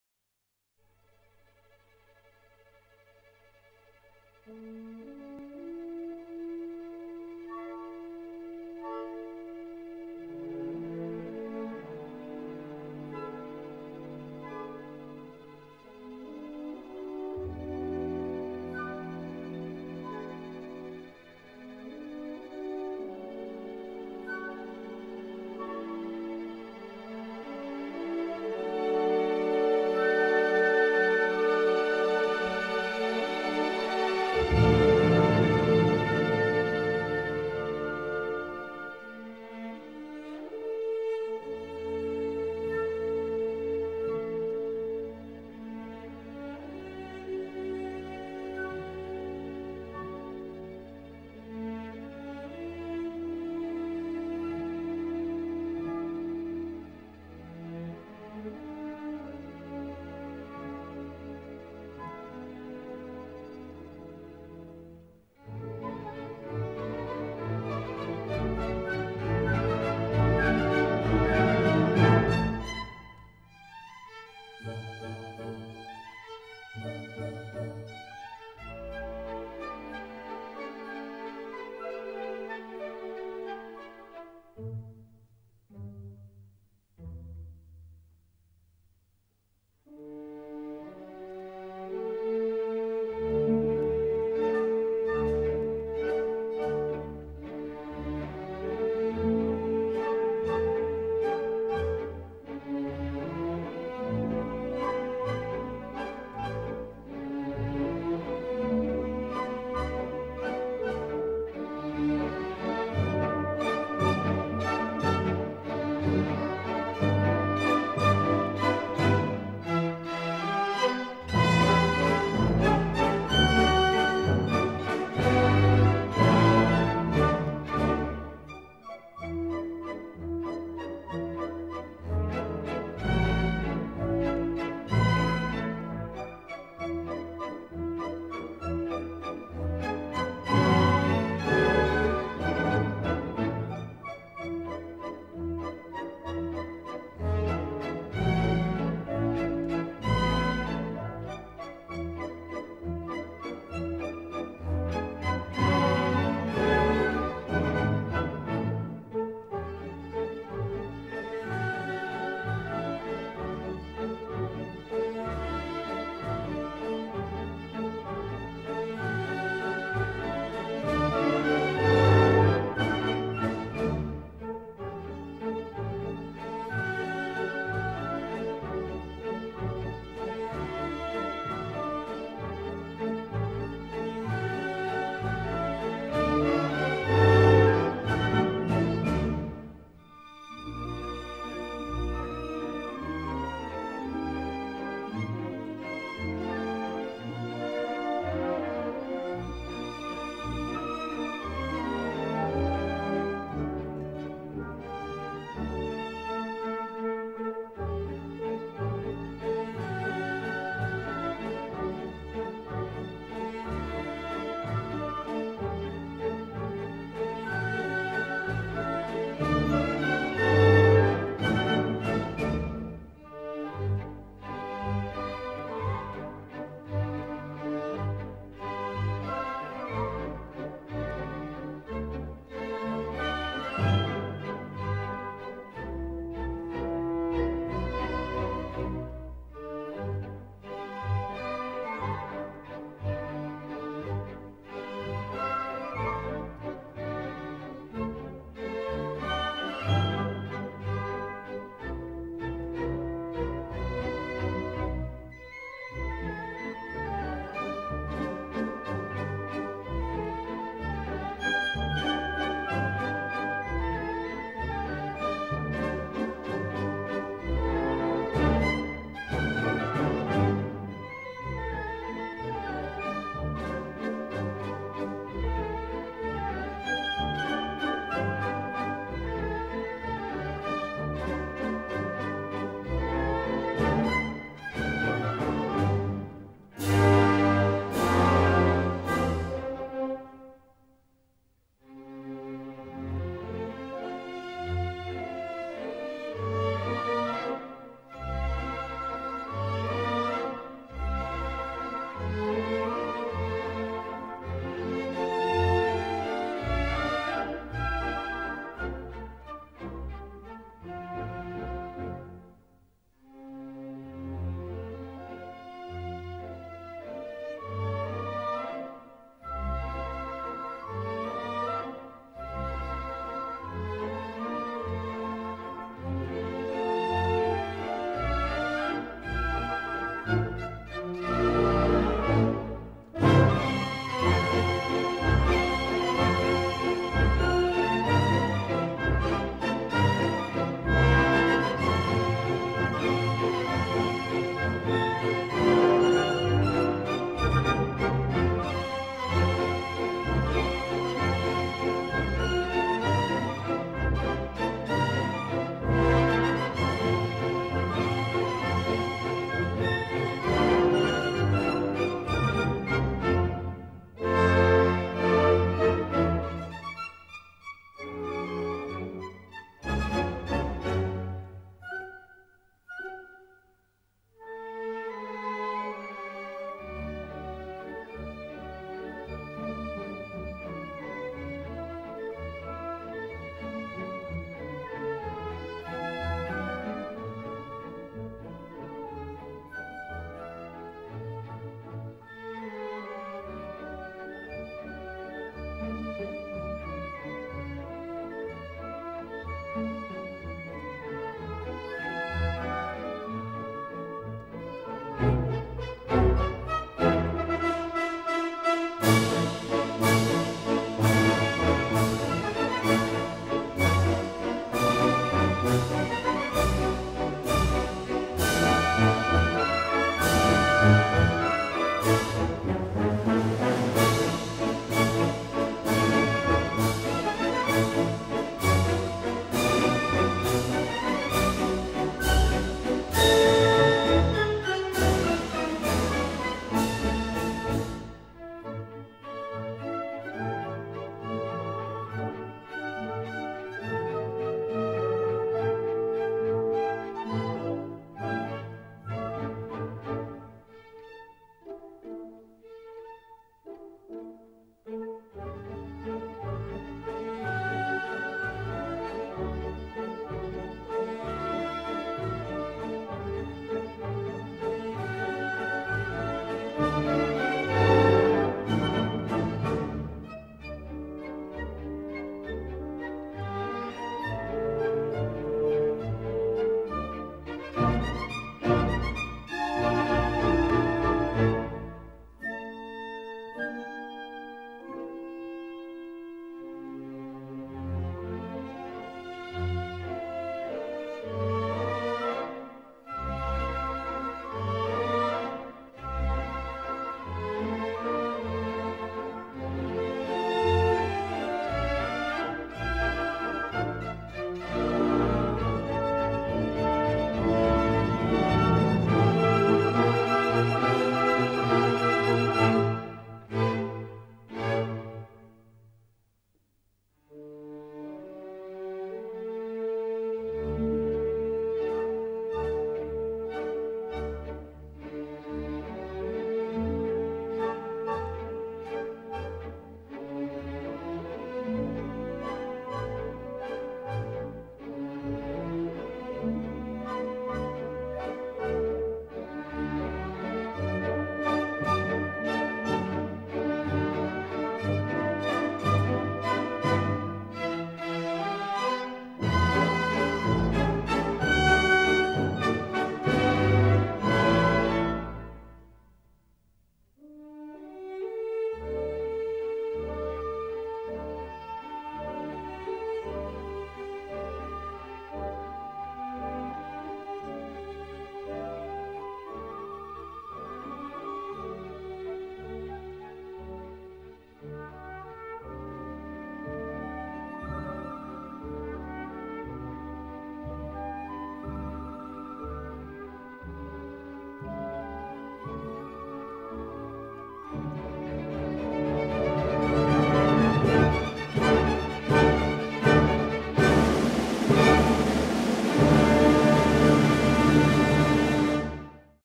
19 Squadron: Waltz at Angels Two-Zero
RAF Spitfires together with French Fighters escort a large French Bomber Raid to the enemy industrial centres in Dusseldorf.